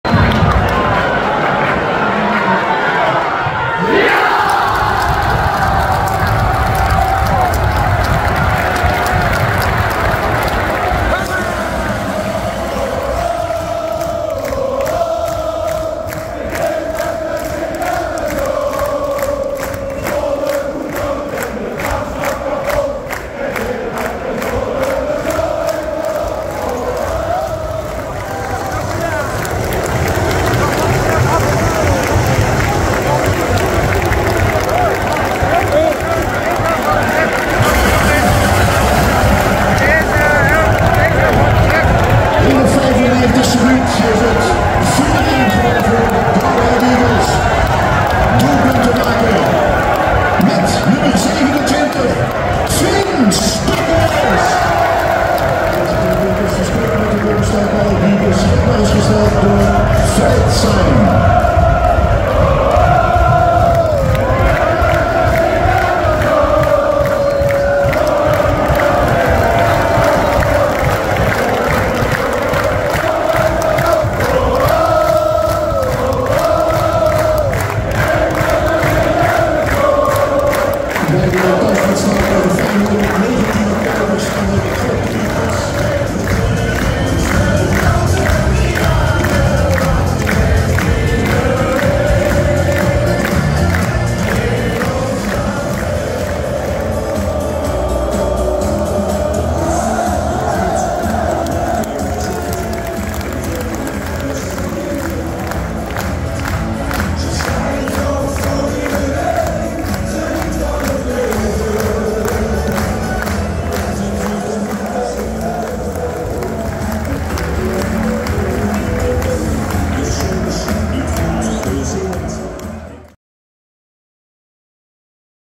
GA Eagles - Heracles 4-1 6-10-2024
En weer drie punten erbij voor GA Eagles. De Leo Halle Tribune viert feest.